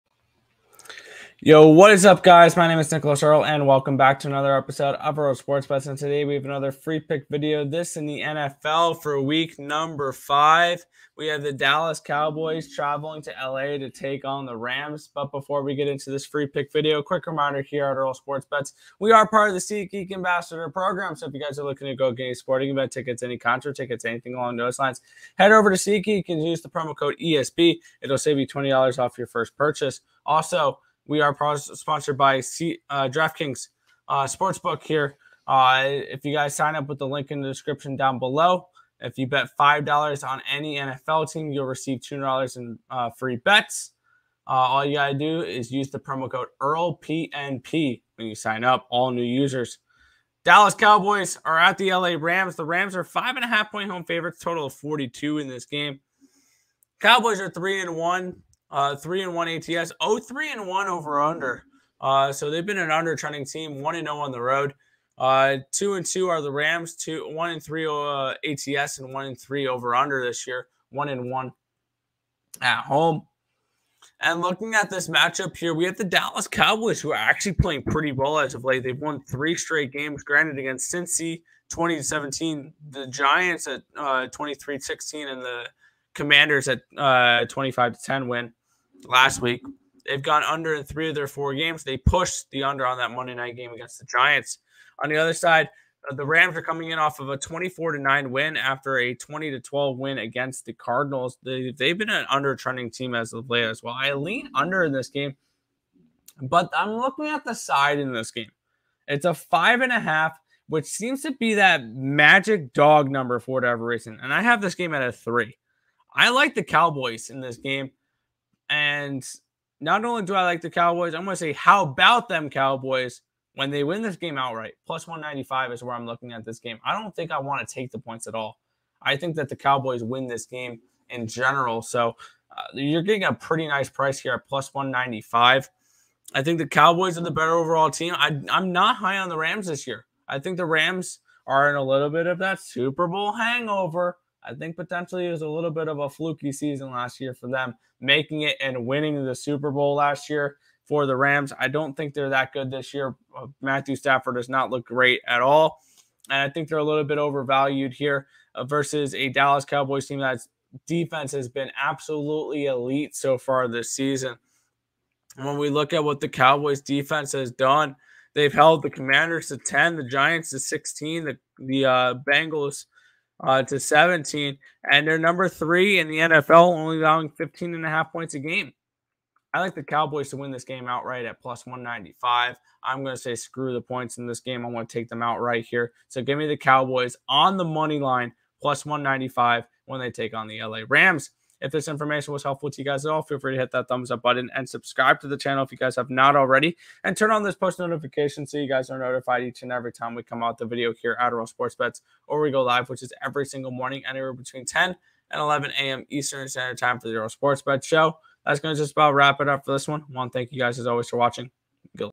NFL Free Pick For October 9th, 2022- Dallas Cowboys @ Los Angeles Rams | Earle Sports Bets
DK Promo- Bet $5 on ANY NFL Team Get $200

Earlesportsbets.com/promo

Picksandparlays.net